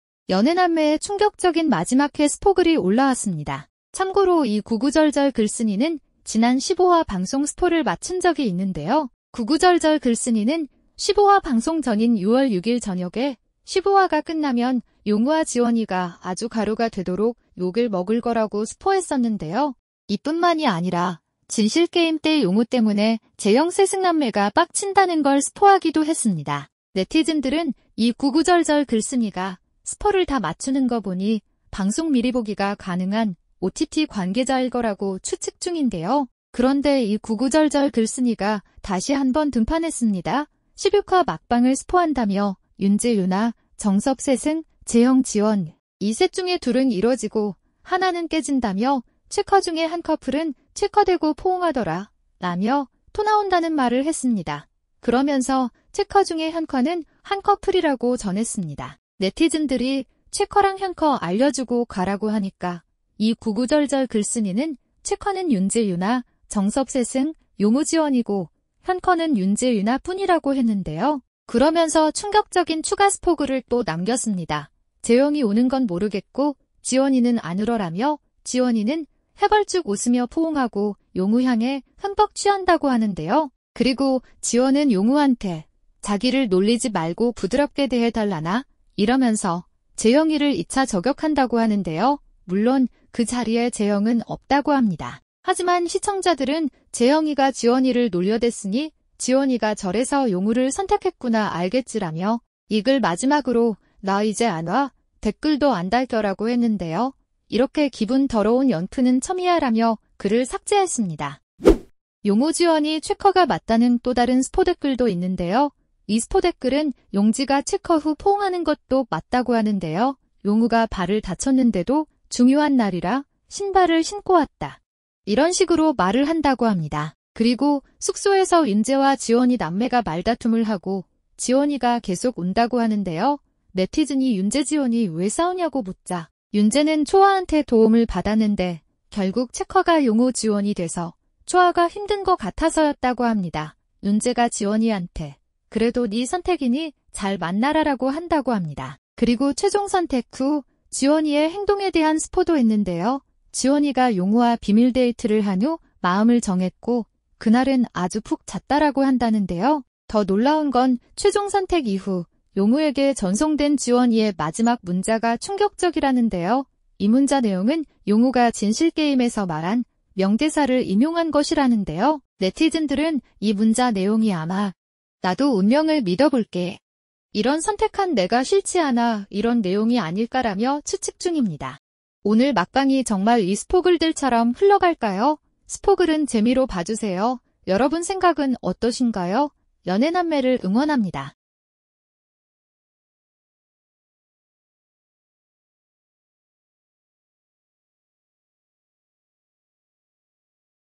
[연애남매] 마지막회 스포, 관계자 폭로, 용우 지원 포옹, 최커는 세커플, 현커는 한커플!
오늘 또 별일잇슈!
매일밤 핫잇슈가 업로드됩니다.💖